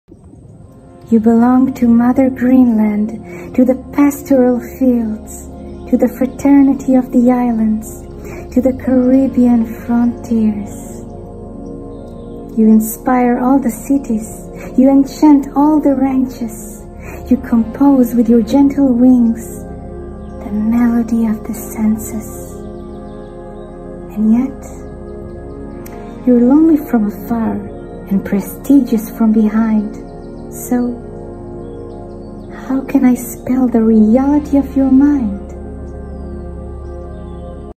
The reality of your mind 🧠
You belong to mother Greenland, to the pastoral fields, to the fraternity of the islands, to the Caribbean frontiers. You inspire all the cities, you enchant all the ranches, you compose with your gentle wings the melody of the senses.
And yet, you’re lonely from afar and prestigious from behind. So, how can I spell the reality of your mind?